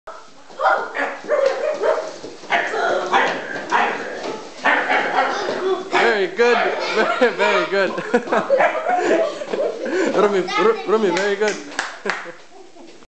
catsanddogs
clip of the children playing dogs and reacting to other dogs and cats